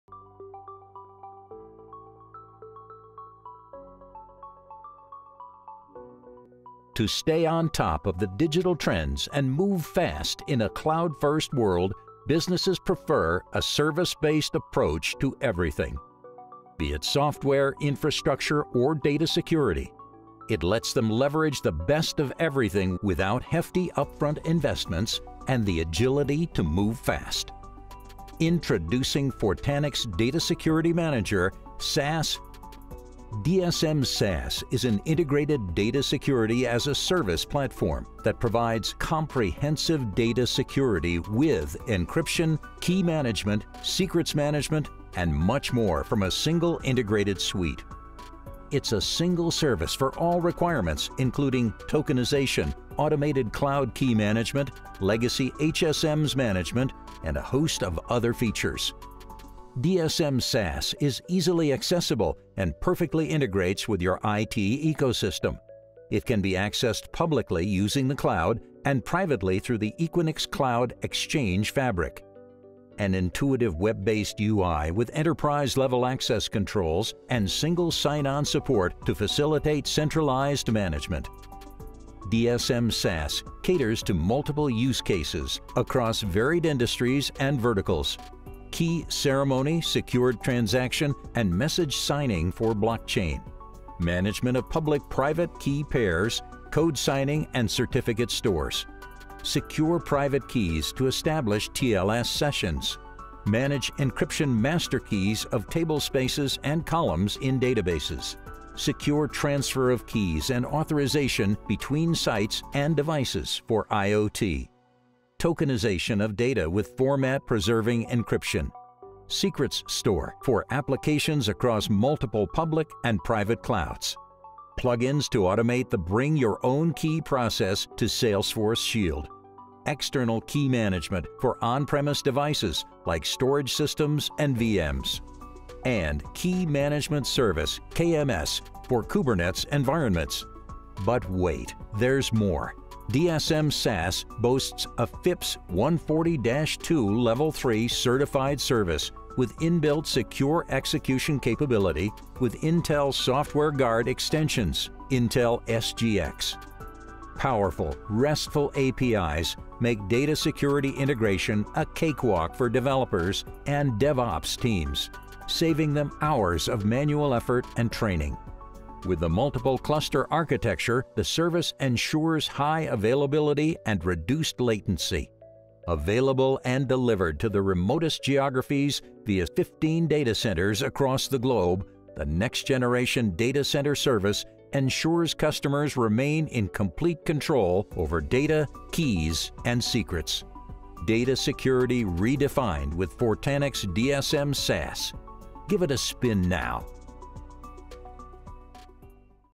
Fortanix Data security Manager SaaS | An Overview
Fortanix Data Security Manager SaaS lifts the limitations of legacy on-premises HSMs with FIPS L3 certified HSMs as a service.

Easy integration to AWS, Azure, and GCP creates a unified security solution across clouds. Your HSM capabilities can now scale alongside your business requirements — on-premises, in the cloud, or hybrid environments.

🔹 Watch our YouTube videos to learn more about confidential computing technology and other data security solutions